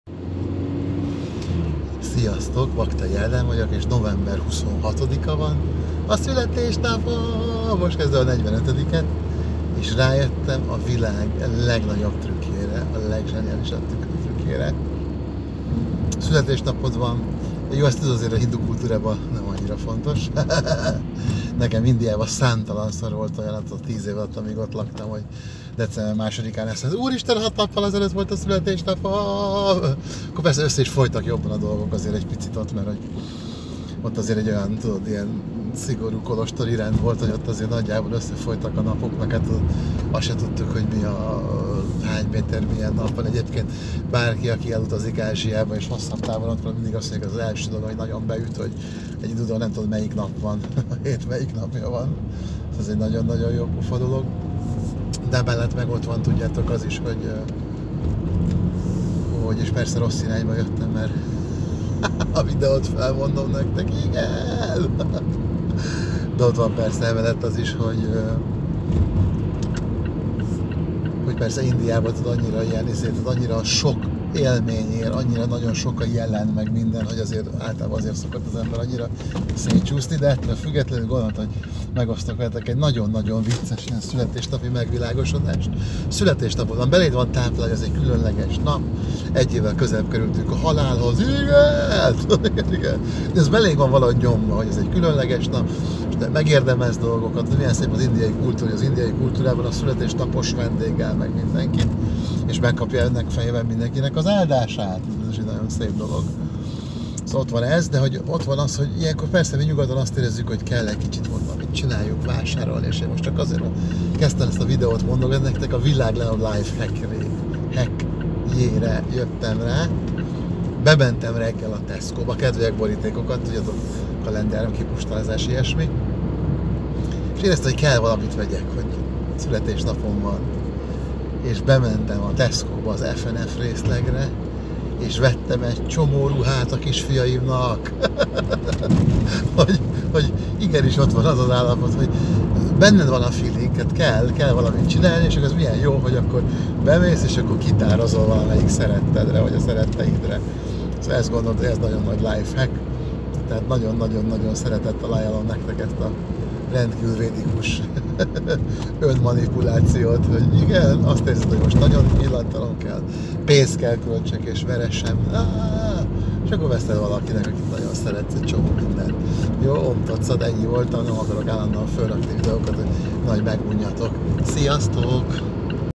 Rájöttem a világ legnagyobb szülinapos life hackjére! Amitől jól érezzük magunkat, mégis önzetlen!
A szülinapom reggelén rájöttem egy szuper szülinapos trükkre! Meg elhangzik ez-az, indiai szokások,  stb.